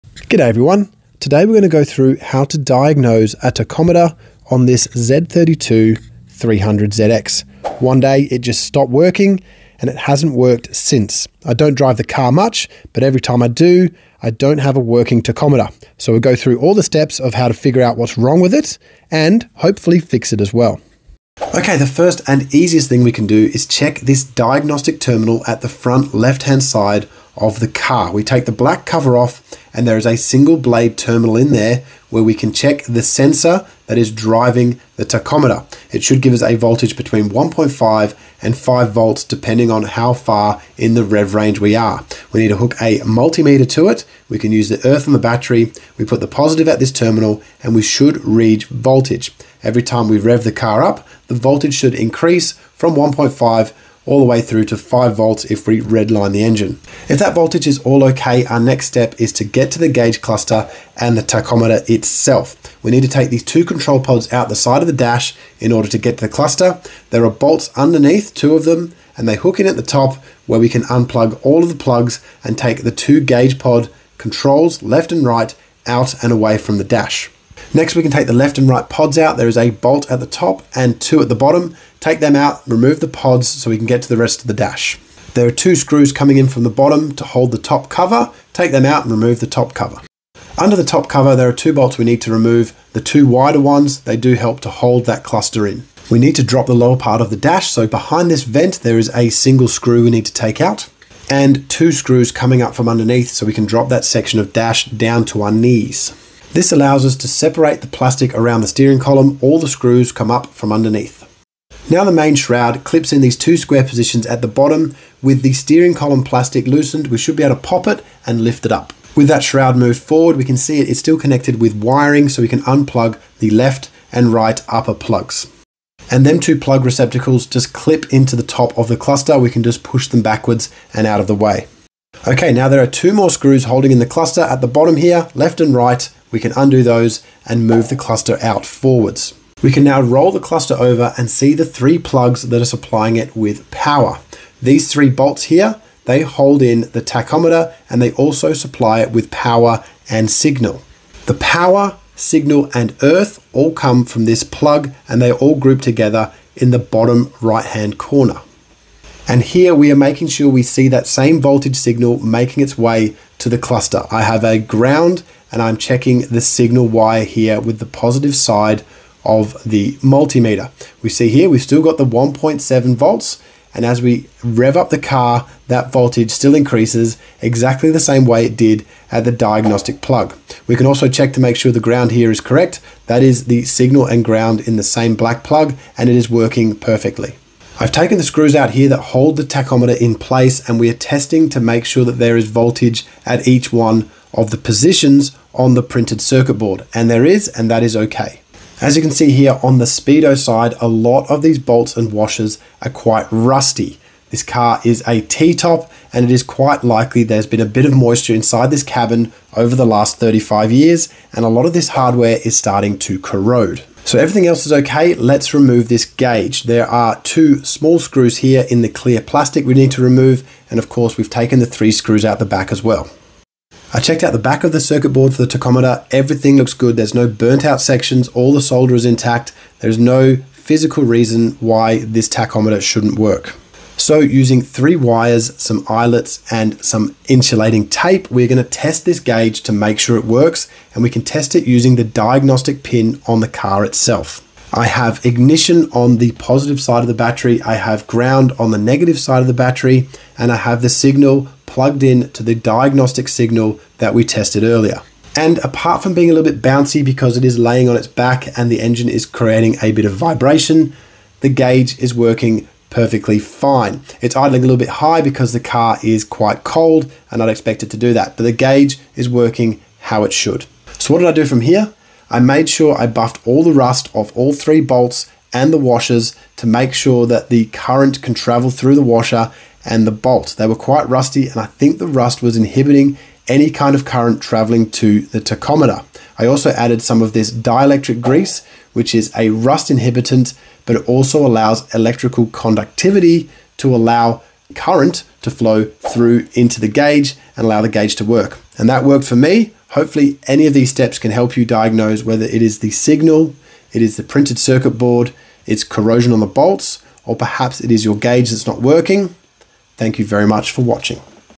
Tachometer Diagnosis (1990s Nissan 300ZX) Step By Step Checks | Rev Counter Diagnosis
How to check all the common issues with these 1990s Nissans, this Z32 300zx had a faulty tacho / rev counter and we fixed the issue through this process.

Nissan Tachometer diagnosis: 00:00
Testing Nissan 300ZX tacho signal: 00:21
Removing 300ZX gauge cluster 01:00
How to test a Nissan 300ZX tachometer 02:20